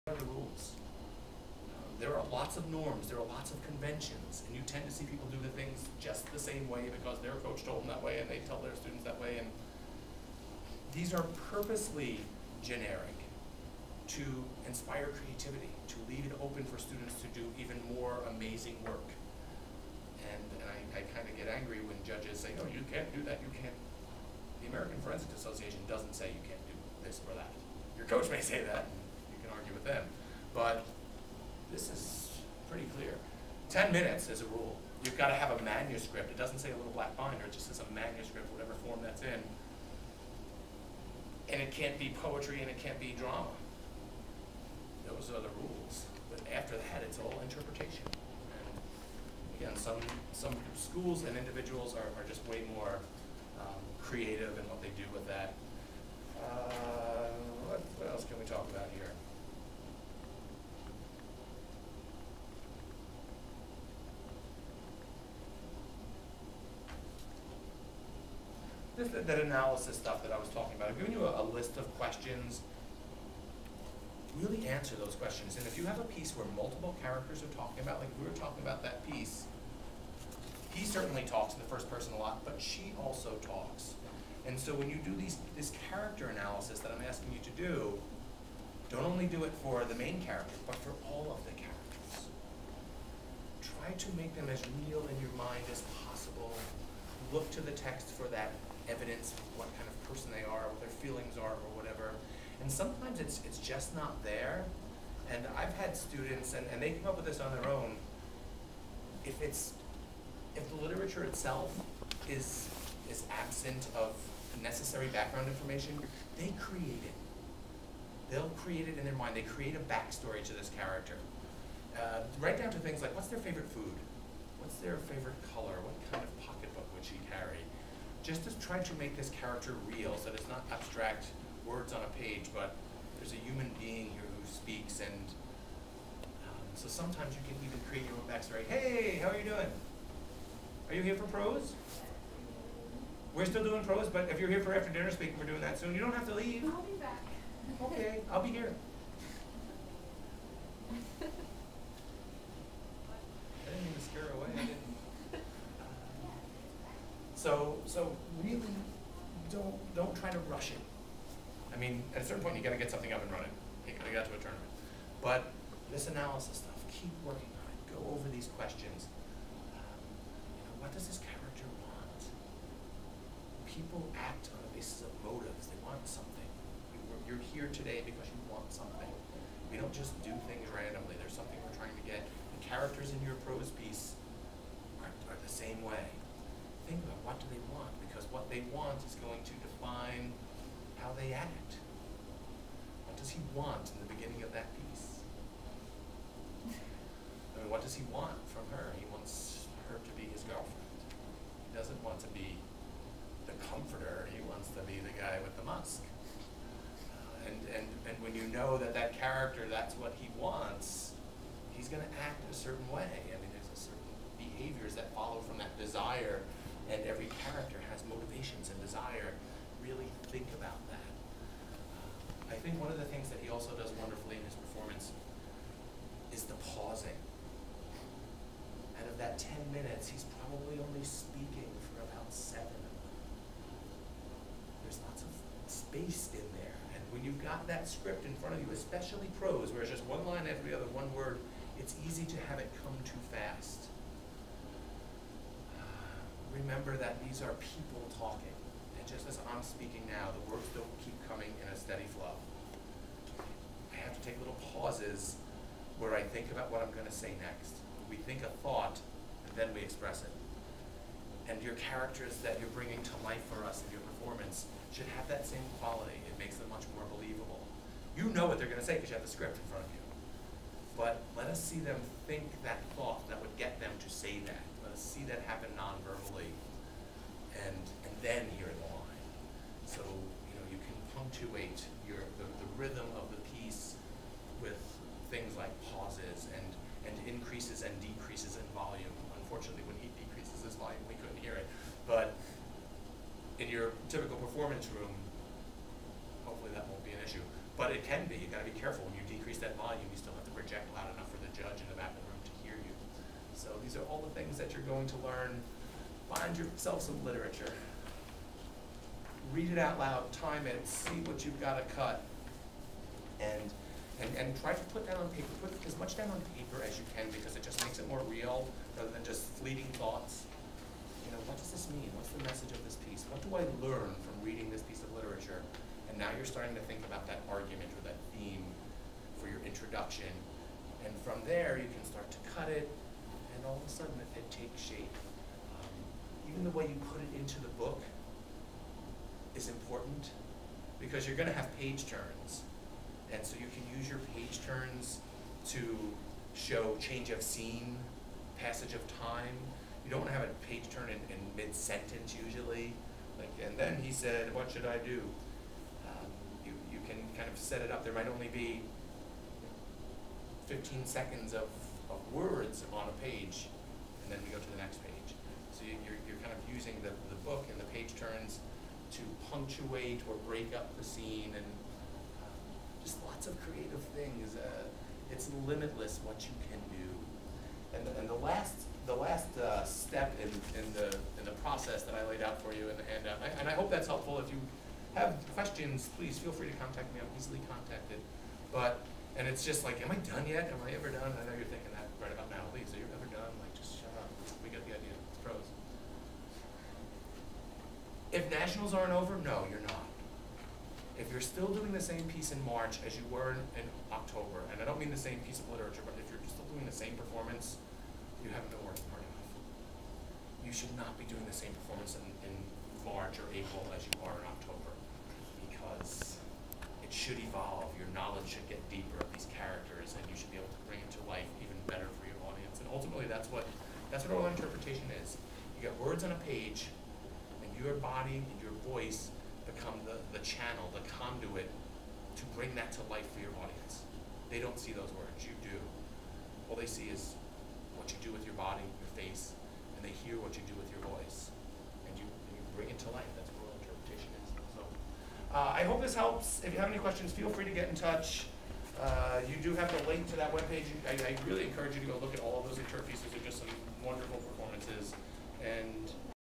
District VII Workshop - Prose, part 5
Dan Leyes of Brookdale Community College gives a lecture on Prose at the 2008 District VII Workshop on the campus of Cedar Crest College.